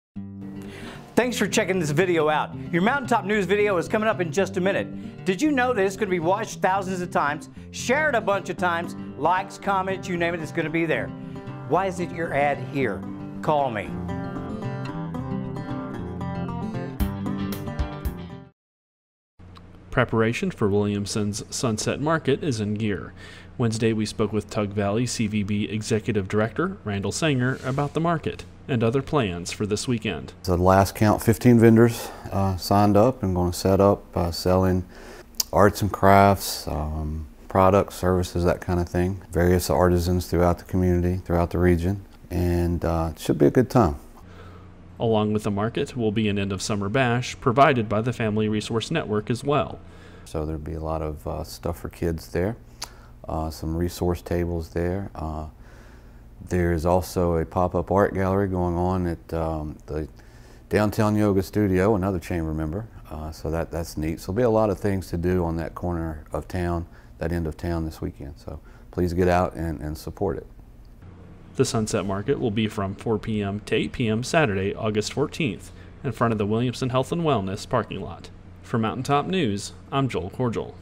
Sunset Market Slated for Saturday in Williamson
Williamson's Sunset Market will take place this weekend.  Times for the market will be Saturday, August 14, from 4 PM to 8 PM.